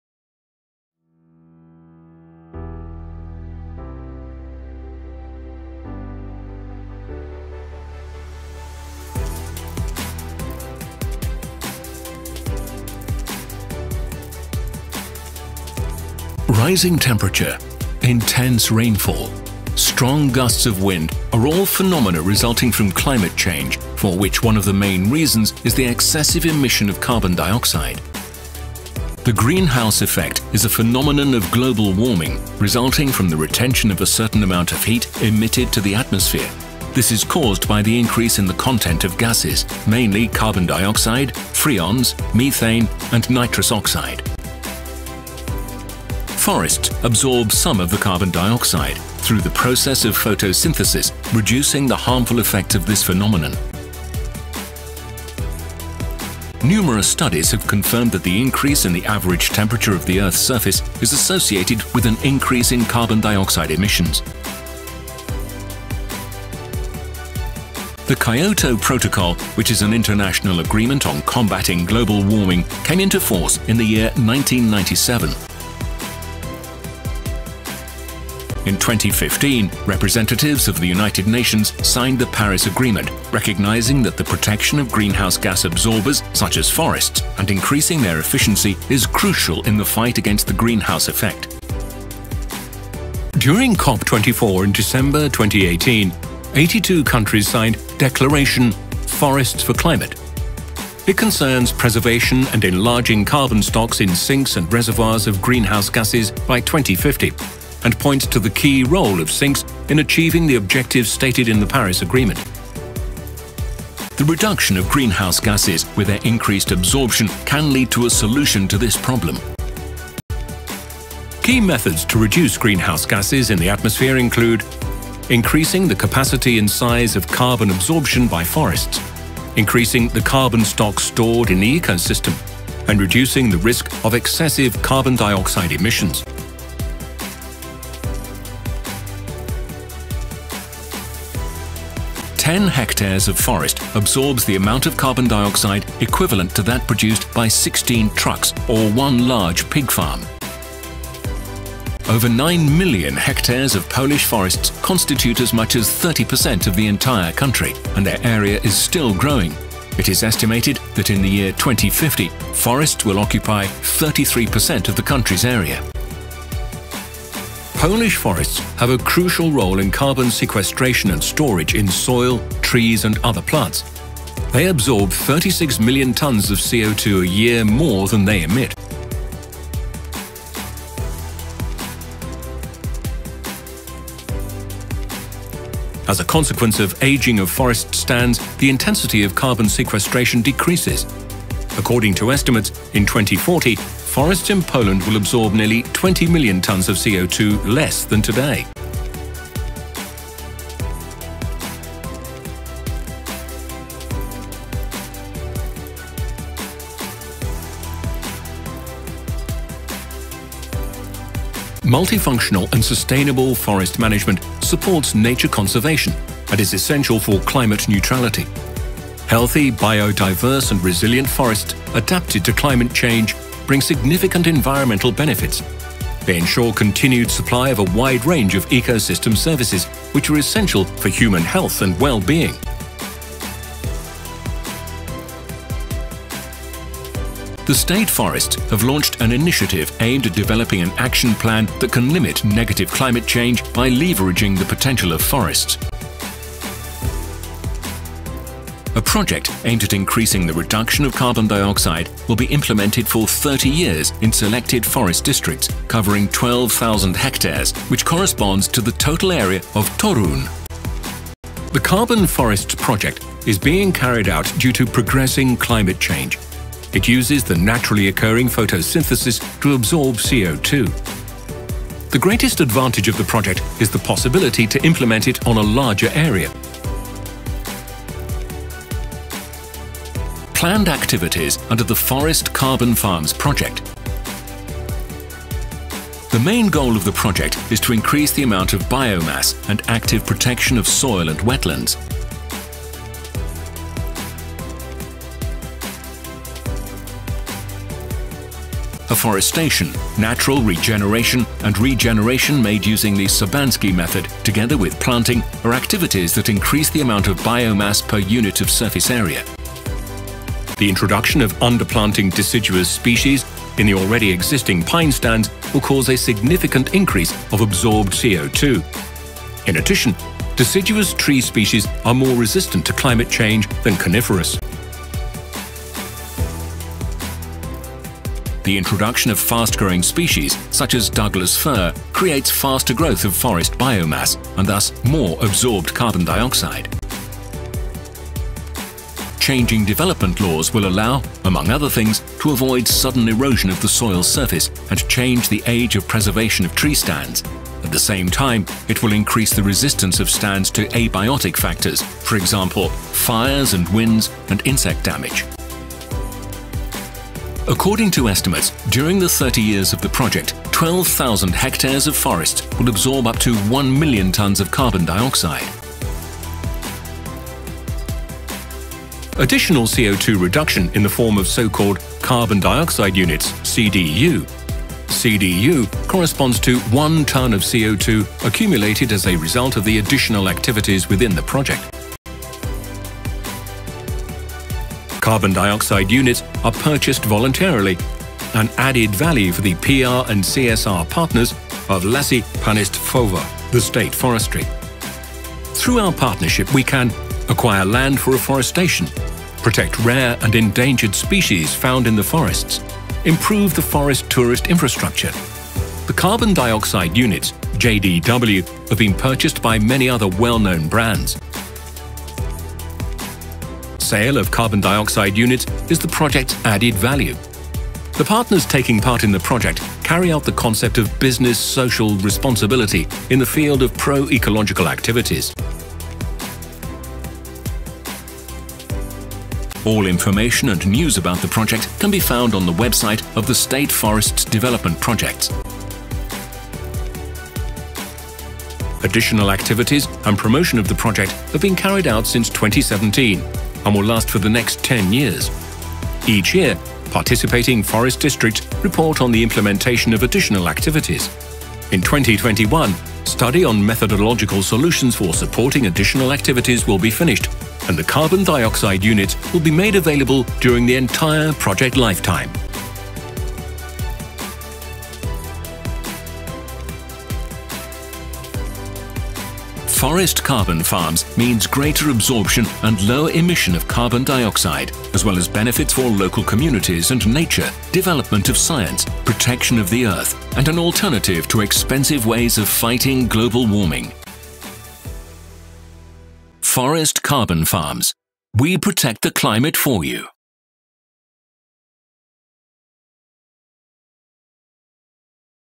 The Forest Carbon Project. What is all about?
The Carbon Forests Project was masterminded as an answer to progressive negative climate changes.